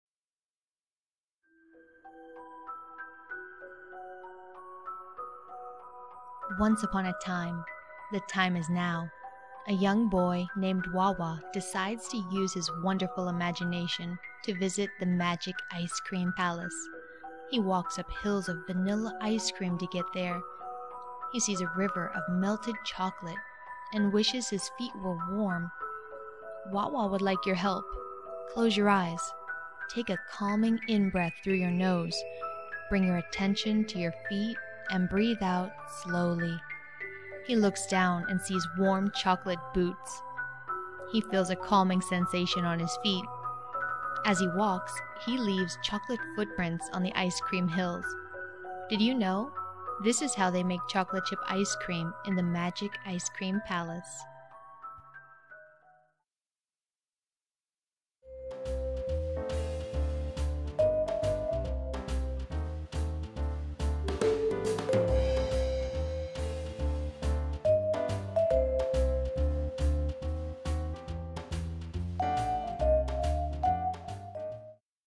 The Magic Ice Cream Palace Excerpt Opening
A children's story for peaceful sleep! Sleep better... tonight!

Help promote sleep health advocacy!